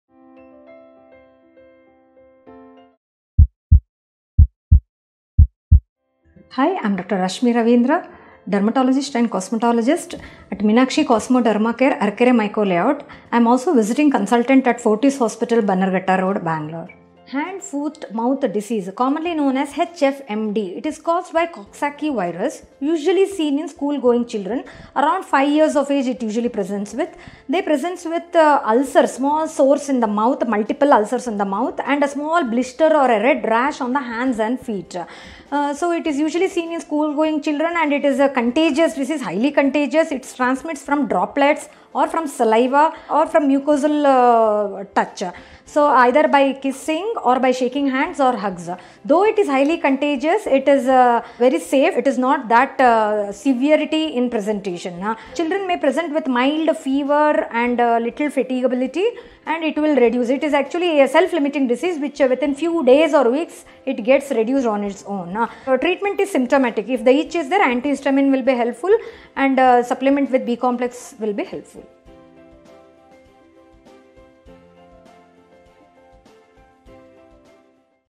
Hand Foot and Mouth Disease in Children | HFMD | Symptoms - Dr. Rashmi Ravindra | Doctors' Circle
Dr. Rashmi Ravindra | Appointment booking No. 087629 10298
Consultant Dermatologist & Cosmetologist |Meenakshi Cosmo Derma Care, Bangalore
Hand foot and mouth disease (HFMD),  it is caused   by Coxsackie Virus.  It is usually seen in school going children.  At around 5 years  of age  it usually presents  with.  They present with small ulcers in   and around the mouth.  There are more   blister or rash on the hands  and feet. It is   usually seen in school going children. It is a contagious diseases highly contagious.  It spreads by droplets, or  form  saliva or from mucosal touch. So either by kissing or by shaking hands. Though it is highly contagious it is  in less severity. It presents with mild fatigability and  it  will reduce. It is a  self  limiting  disease.